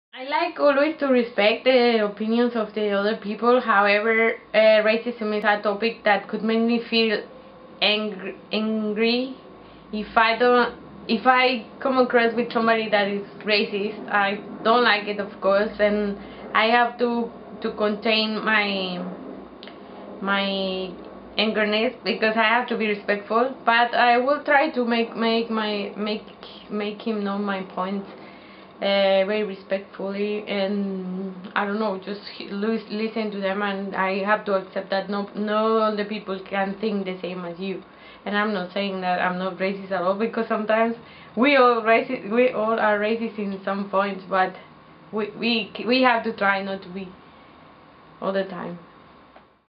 Talk About Racism: Paulina - How would you discuss racism with somebody who has a different opinion?
All Together Now asked Paulina how she would discuss racism with somebody who has a different opinion to herself.

This interview was filmed for our "Talk About Racism" project

Tell us what you think about our video